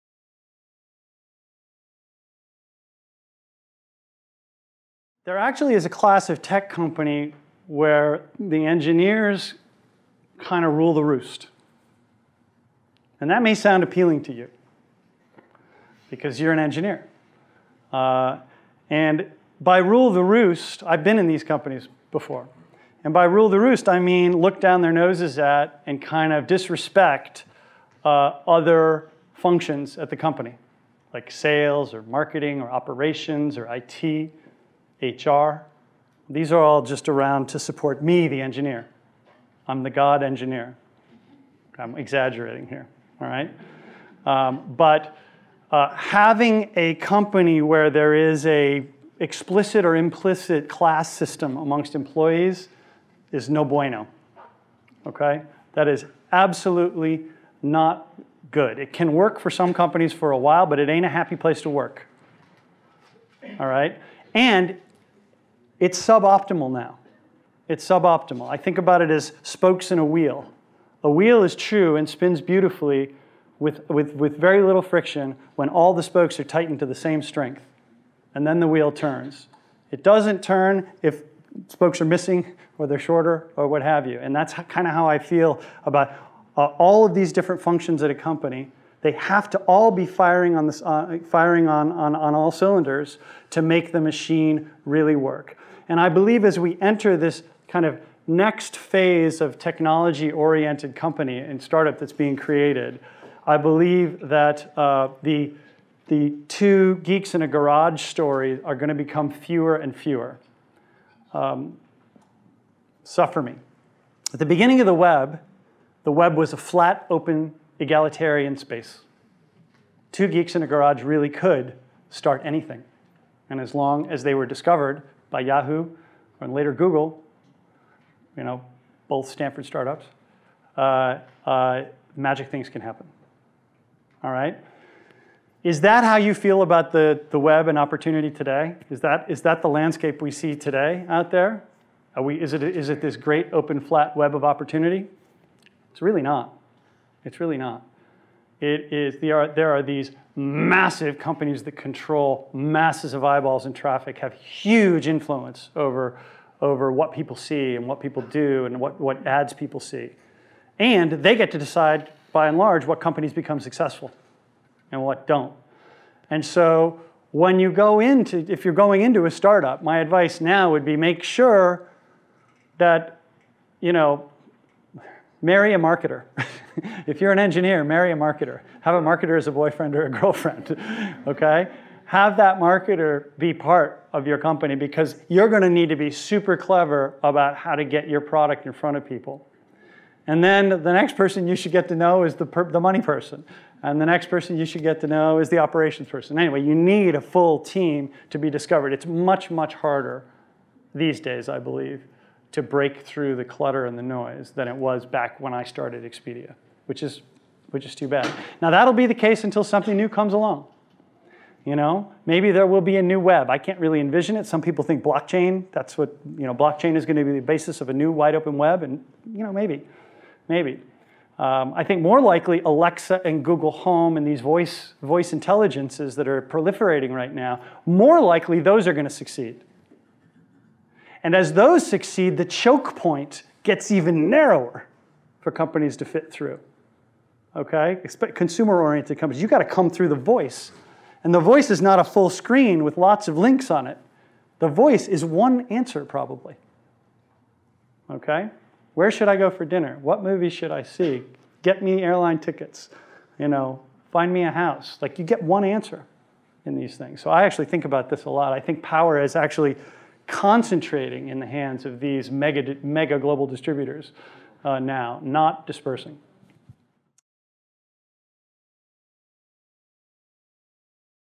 Rich Barton: Engineering Isn’t Everything
Zillow Group and Glassdoor Co-Founder Rich Barton explains how tech companies that hold engineers in higher regard than employees in other areas of the organization have an outdated understanding of the Internet. It’s no longer a place just for the tech savvy, and rising above the online chatter requires experts in marketing, finance and other facets of business, according to Barton.